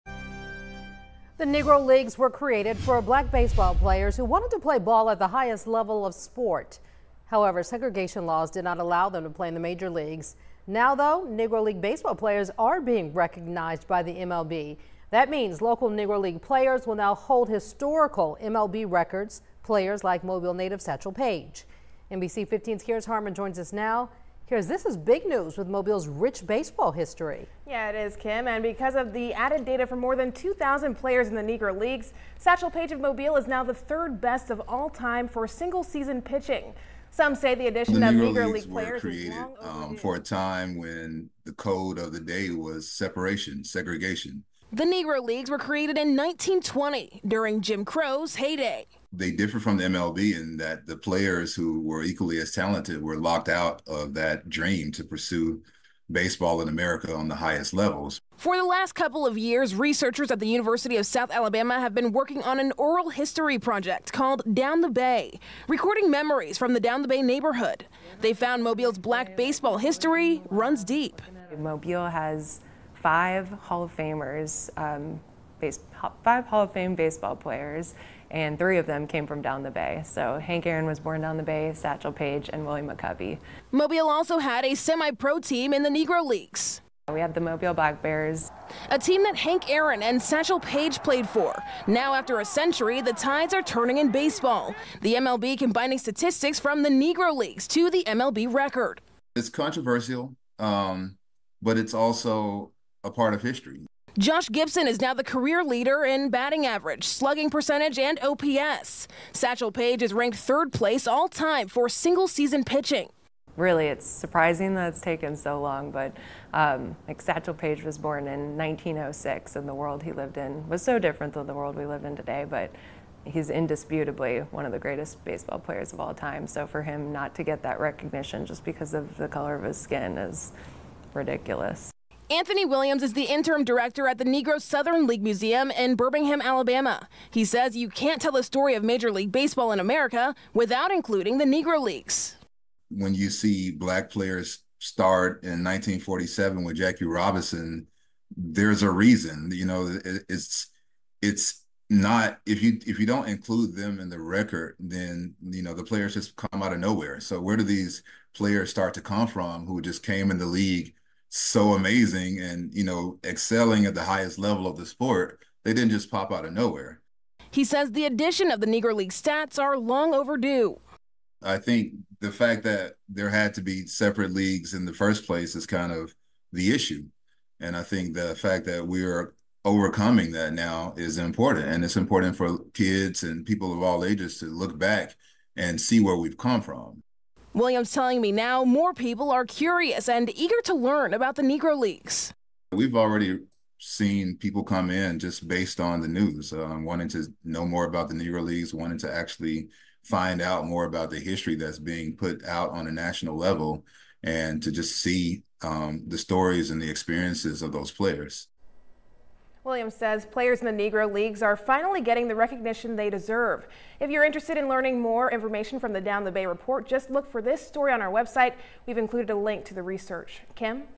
Negro League legends like Mobile's Satchel Paige now recognized as MLB record holders - NBC 15 WPMI
Satchel Paige is ranked 3rd place all time for single season pitching.